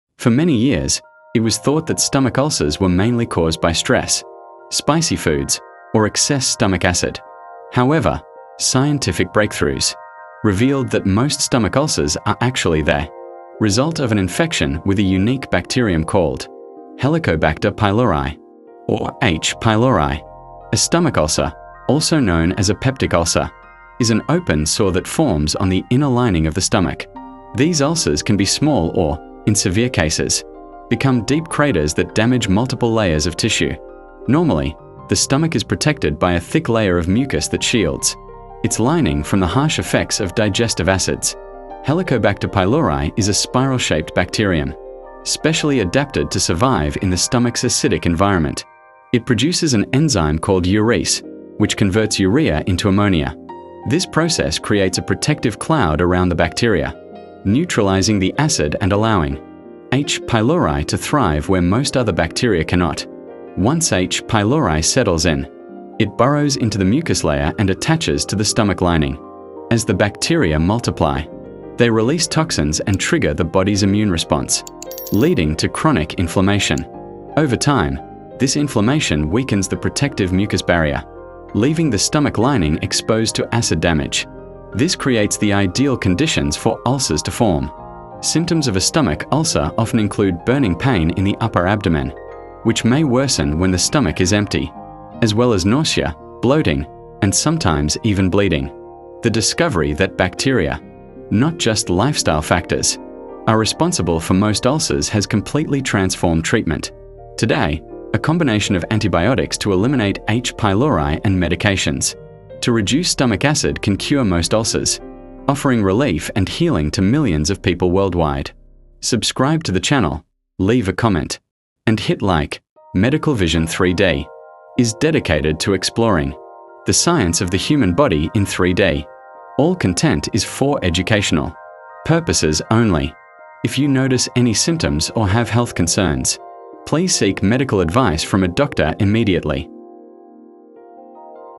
Stomach Ulcer Symptoms You Should NEVER Ignore!
🔬 Discover the Real Cause of Stomach Ulcers in 3D Animation! 🔬

Think stress or spicy food causes ulcers? Think again! Dive into our detailed 3D medical animation and see how the tiny but powerful Helicobacter pylori bacteria 🦠 invades your stomach and leads to painful ulcers.

What you’ll learn in this video:
What a stomach ulcer is and how it forms 🫀
The role of H. pylori in ulcer development
Close-up views of bacteria attacking the stomach lining
Key symptoms to watch for ⚠️
Modern treatments that can cure most ulcers 💊

🌍 Perfect for everyone: students, healthcare professionals, or anyone curious about digestive health and science!

📈 Why watch?
High-quality, accurate 3D medical visuals
Easy-to-understand explanations
Science made simple for a worldwide audience